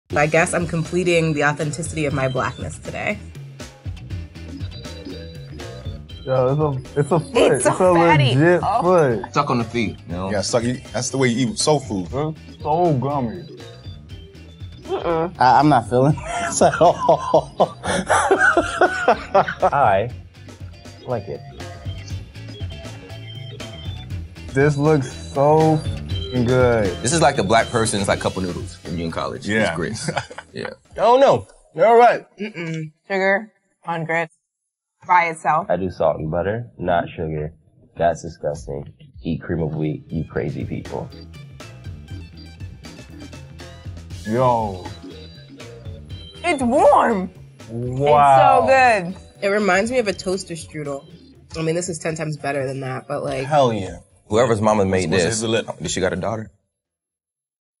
Black People Try Soul Food For The First Time
“It’s so gummy!”